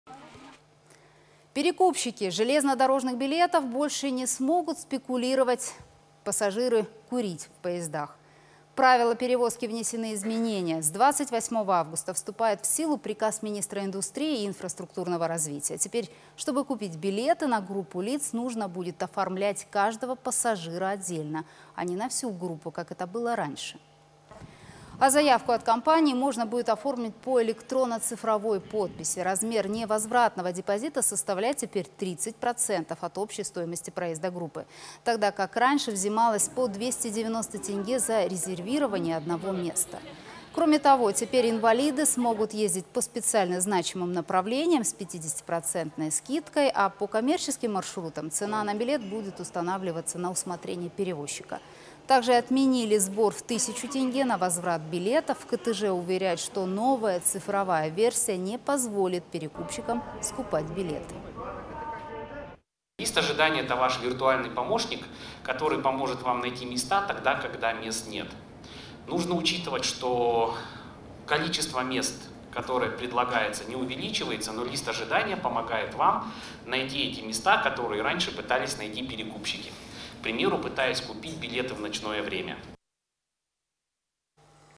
Перекупщики железнодорожных билетов больше не смогут спекулировать
Перекупщики железнодорожных билетов больше не смогут спекулировать, а пассажиры – курить в поездах. В правила перевозок внесены изменения. С 28 августа вступает в силу приказ министра индустрии и инфраструктурного развития. Теперь, чтобы купить билеты на группу лиц, нужно будет оформлять на  каждого пассажира отдельно, а не на всю группу, как это было раньше.